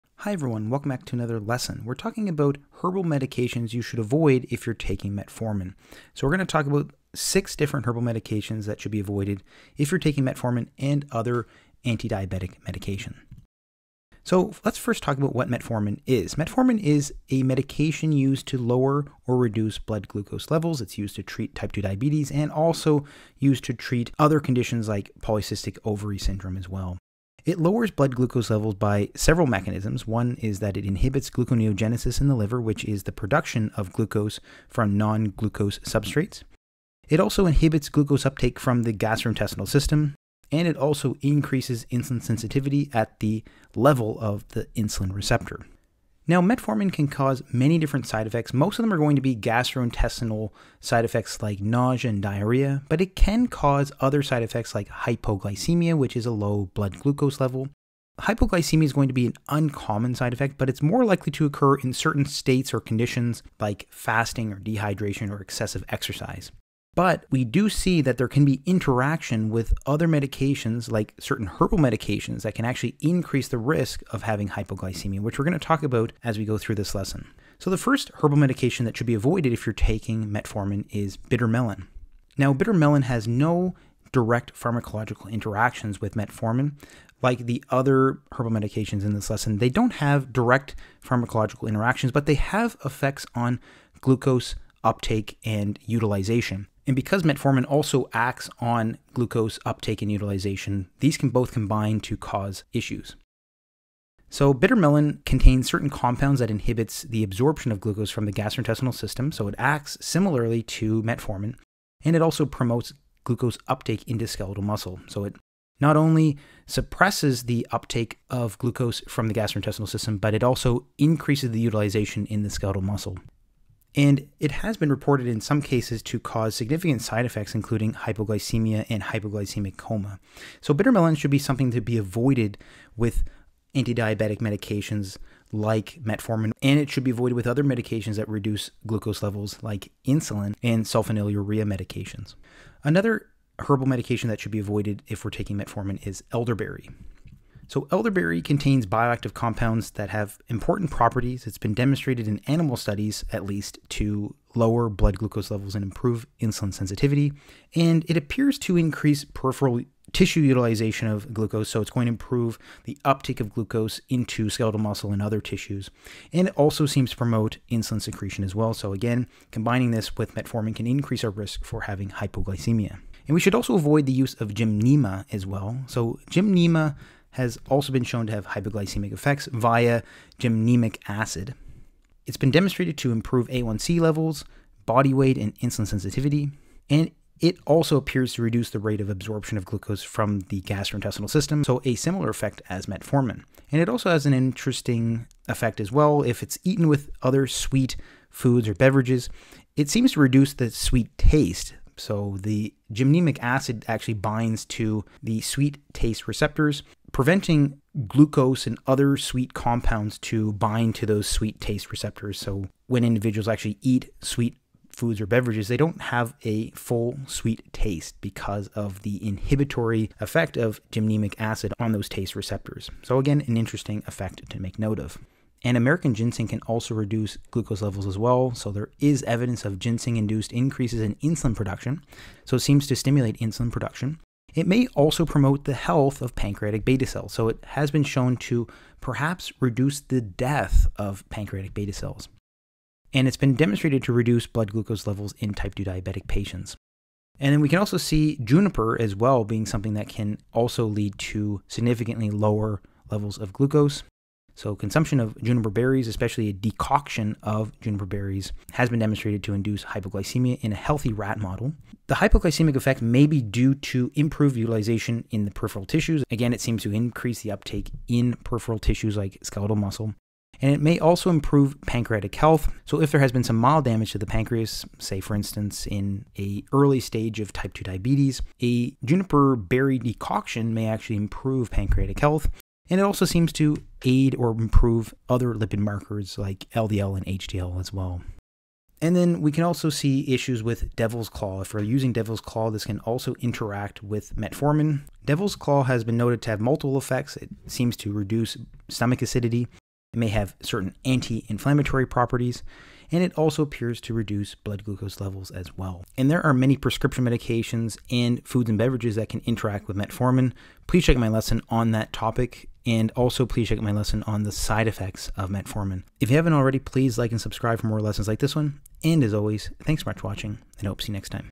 Metformin & Herbal Medications | 6 Herbal Interactions to Avoid
Metformin is a medication used to reduce or lower blood glucose levels, which is useful for treating type 2 diabetes and also helping resolve symptoms of polycystic ovary syndrome. Metformin reduces blood glucose levels via at least 3 mechanisms, including reducing gluconeogenesis in the liver, reducing absorption of glucose from the gastrointestinal tract, and also increasing activation of the insulin receptor (increases insulin sensitivity). In this lesson, we discuss 6 herbal medications you should avoid when taking metformin.

JJ

For books and more information on these topics